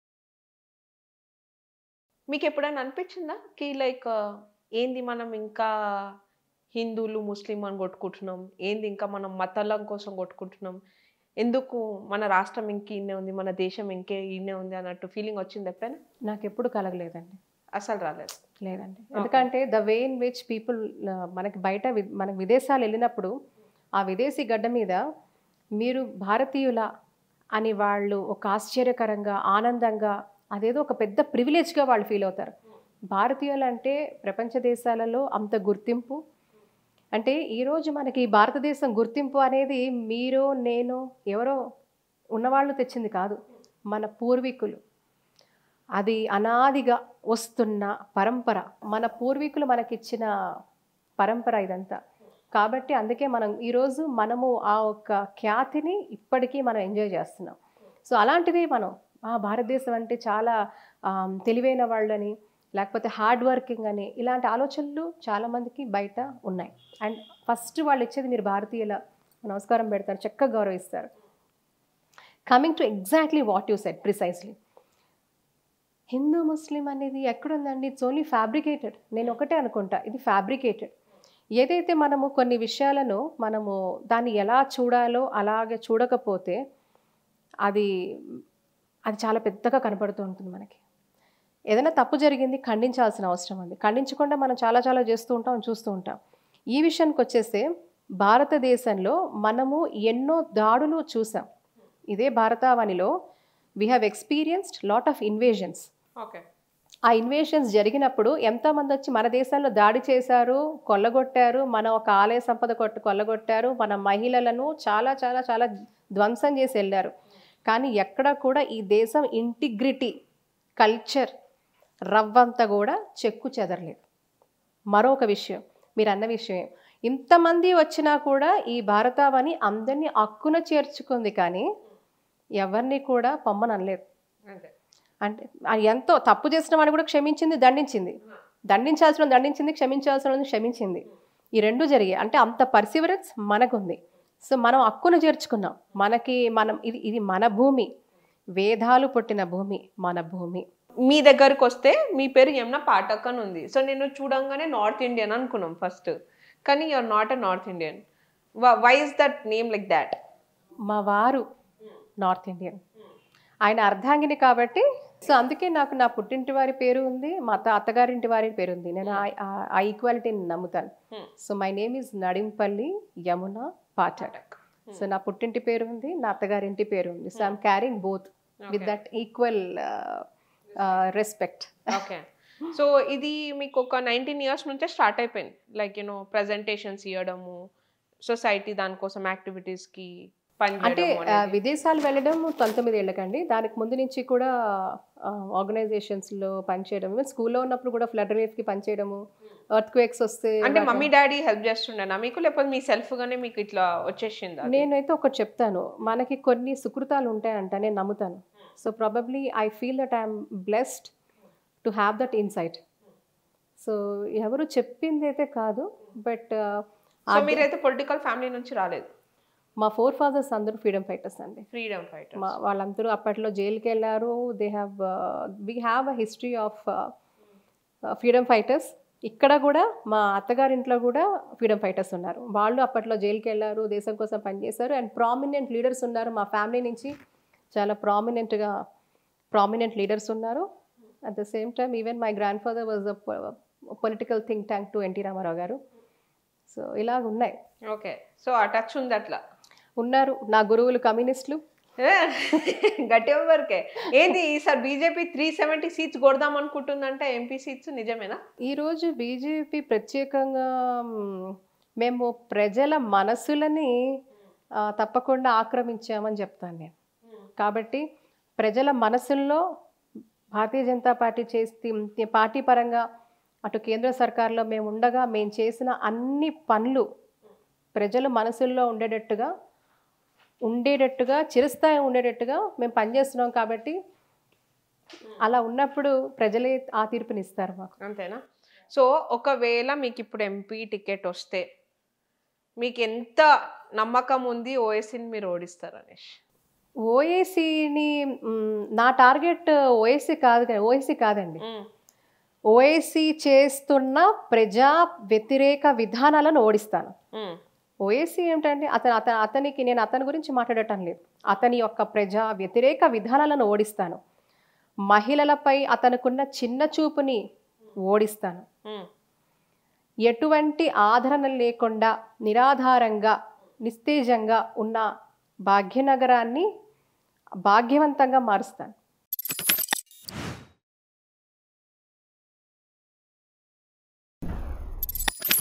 ఒవైసీని ఓడిస్తాం..?🔥 | BJP Leader Yamuna Pathak About Asaduddin Owaisi | MP Elections 2024 | Mic TV
ఒవైసీని ఓడిస్తాం..?🔥 | BJP Leader Yamuna Pathak About Asaduddin Owaisi | MP Elections 2024 | Mic TV New